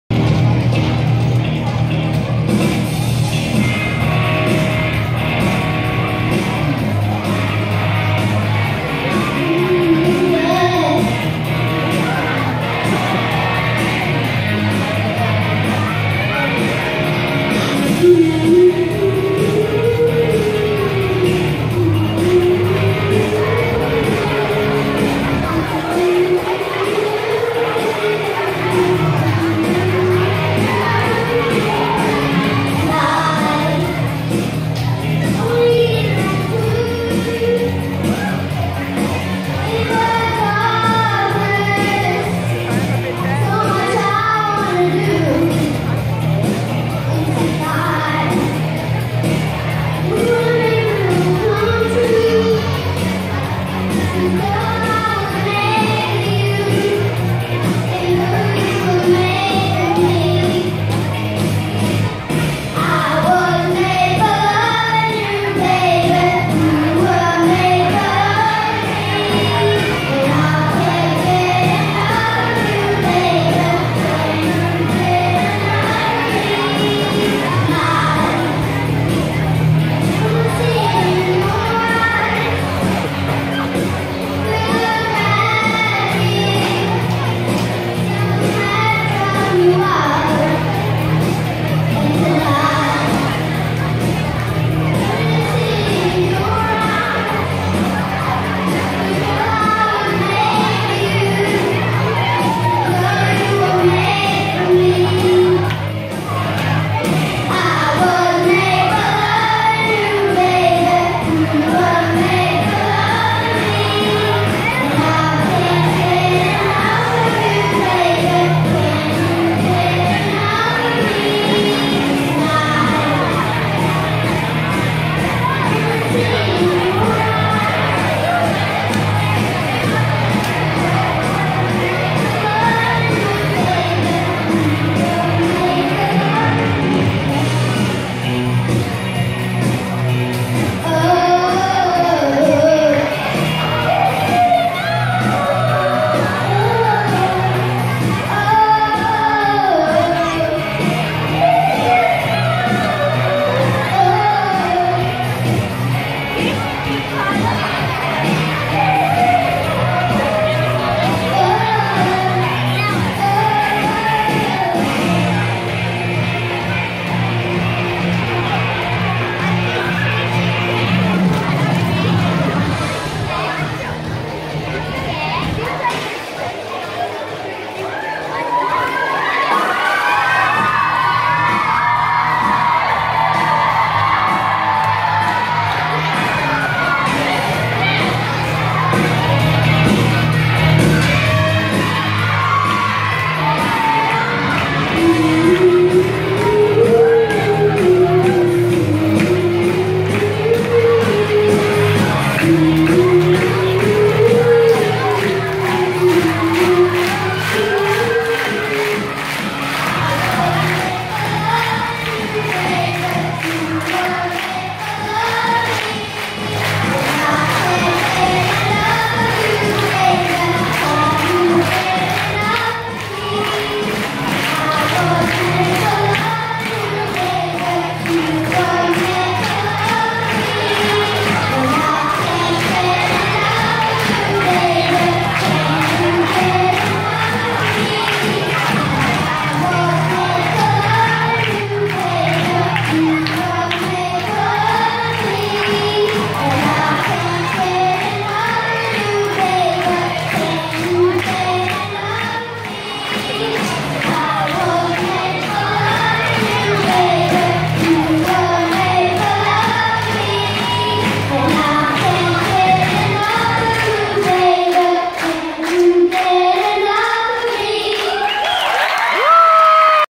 Emu Park Talent Show 2017
Awesome effort from these little guys at the school talent show - Fletcher, Angus, Barney and Percy